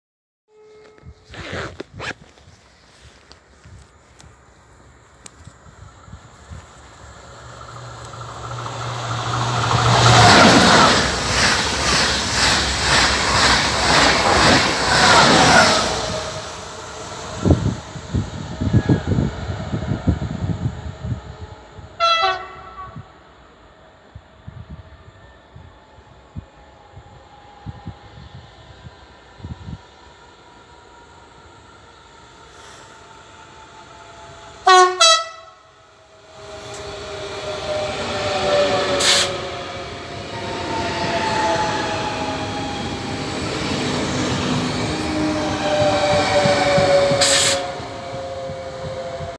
Croisement entre TGV et Ter 2nng
TGV 3UH Euroduplex rame 805  pour Grenoble et un 2nng pour St-André-le-gaz